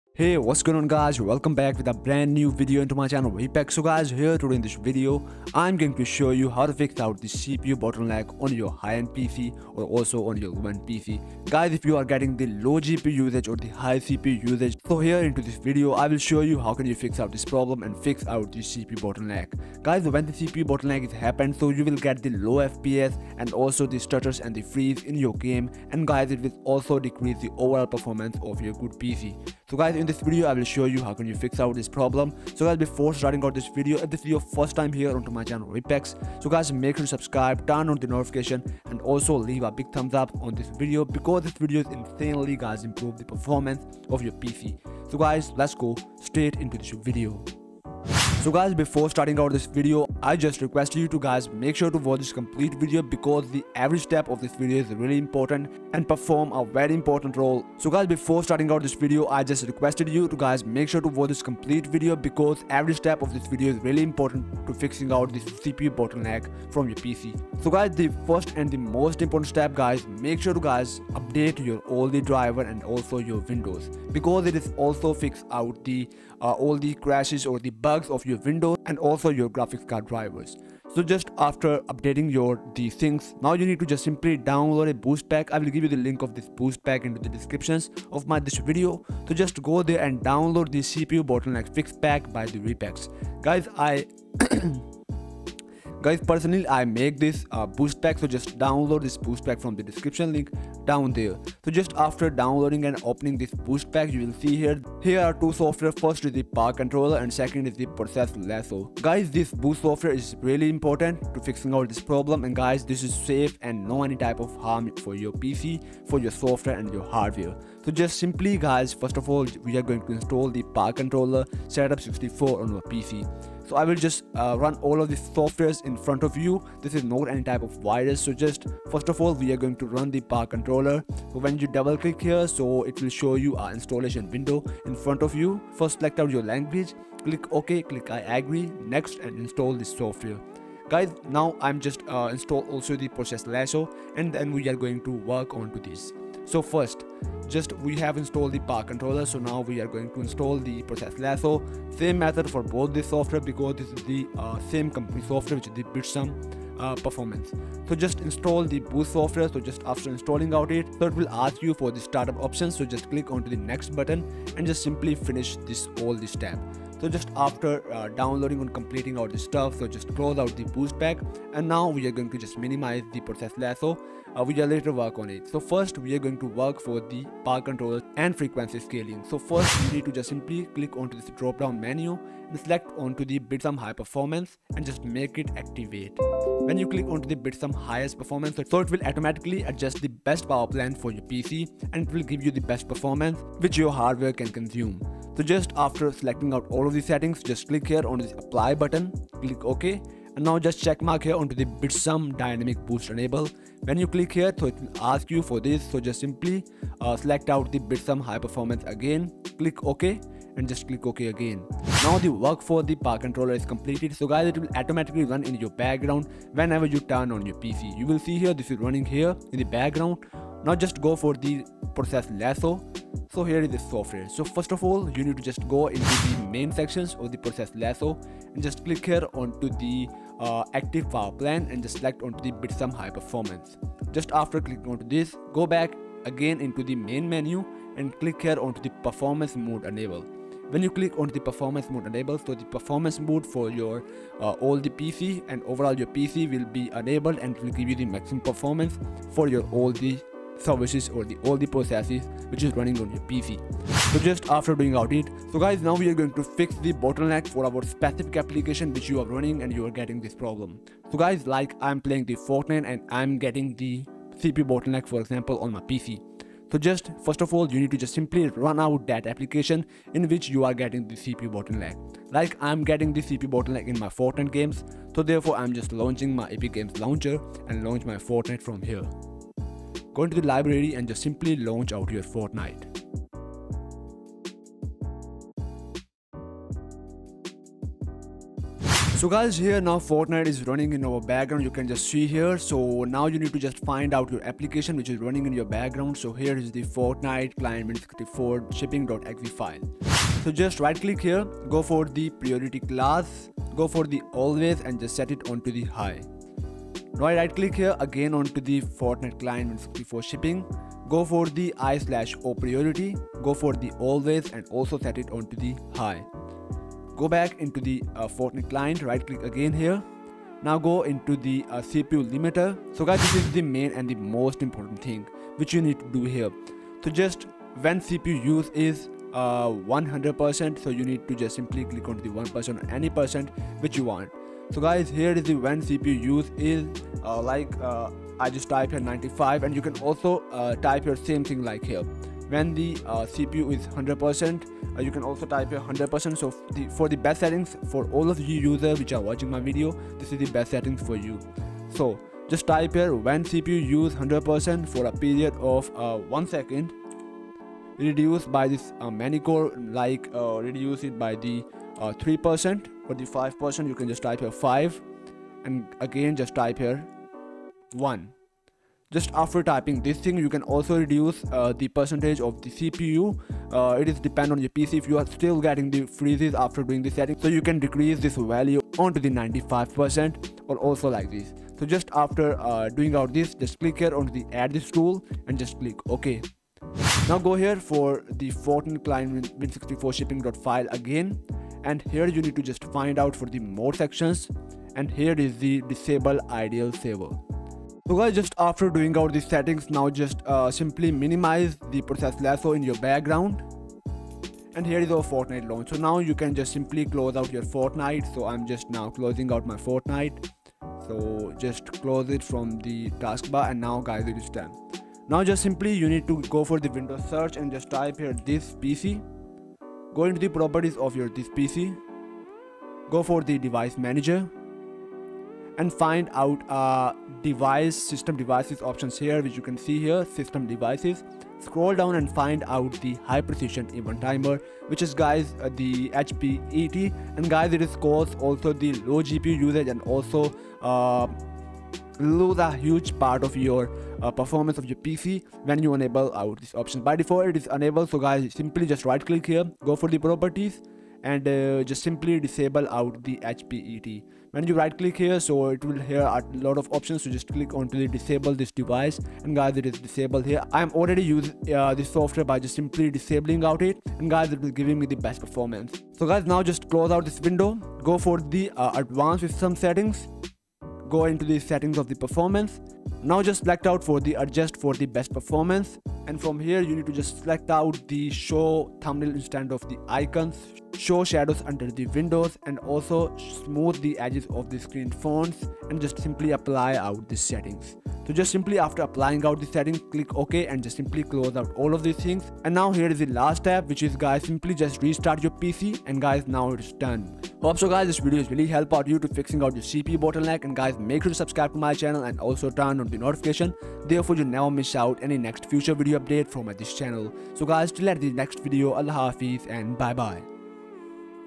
How To Fix CPU Bottleneck (Fix Stutters/Freezing) | Increase GPU Performance
This Video Will Teach You To

In this video i show you some simple tweaks/setting to fix low gpu usage which is actually happened because of cpu bottlenecking and loose huge performance which also cause low fps/stutters and freezing in games.

Make Sure To Watch Complete Video